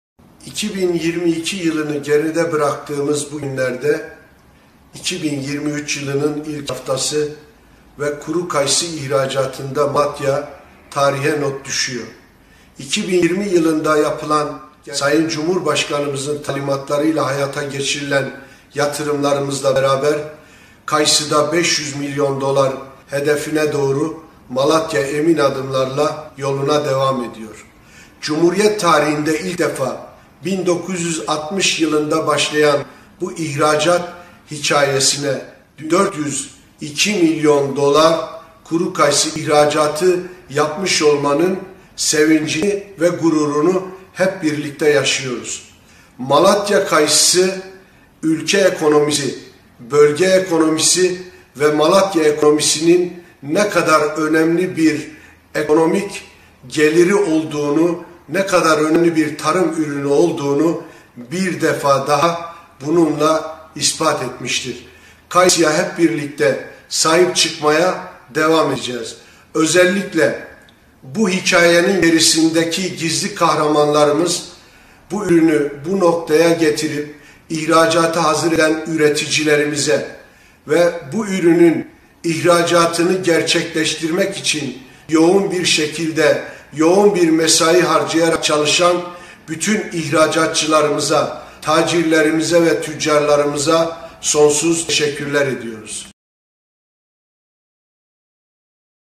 Kayısı ihracatı rekor kırdı
Malatya Ticaret Borsası Başkanı Ramazan Özcan, 2022 yılında toplam da 76 bin 355 ton kuru kayısı ihracatı gerçekleştirildiğini bunun karşılığında 402 milyon dolar gelir elde edildiğini açıkladı. Özcan, iç piyasa ve kayısı çekirdeği ile birlikte kayısı ticaretinden Malatya’nın şu anda 500 milyon dolar gelir elde ettiğini aktardı.

Ticaret Borsası Başkanı Ramazan Özcan, basın toplantısı düzenleyerek kayısı ihracatı ile ilgili 2022 yılı rakamlarını açıkladı.

-“İlk defa 402 milyon dolar da gelir elde edildi”

Ticaret Borsası Başkanı Ramazan Özcan, “Malatya'da 1980li yıllarda kayıt altına alınan kayısı ihracatında ilk defa bir rekor kırıyoruz. 2022 yılında da 76 bin 355 ton kuru kayısı ihracatı gerçekleştirdik ve cumhuriyet tarihinde, 1960 yılında başlayan ihracat hikayesi ile birlikte ilk defa 402 milyon dolar da gelir elde edildi. Kuru kayısı ihracatı ile beraber çekirdek ihracatımız ve bununla birlikte iç piyasa diye adlandırdığımız Türkiye sınırları içerisindeki tüketimle beraber yaklaşık 500 milyon dolar gelir elde edilmiş durumda.” şeklinde bilgi verdi.

-“2023 yılı ihracat hedefi 500 milyon dolar”

2023 yılının da kayısı için bir atılım yılı olacağını belirten Özcan şöyle konuştu, “2020 yılında hayata geçirmiş olduğumuz Kuru Kayısı Lisanslı Depoculuğu ile beraber Türkiye Ürün İhtisas Borsacılığı ve elektronik satış sistemini de entegre ederek artık kayısıyı bir yatırım aracına dönüştürmeyi planlıyoruz. 2023 yılı 2022 yılında olduğu gibi kuru kayısı tarımda Malatya için bir atılım yılı olacak. 2023 yılında 100 bin ton ihracat hedefimizi ve 500 milyon doların üzerinde bir ihracat hedefleyerek yolumuza devam edeceğiz.”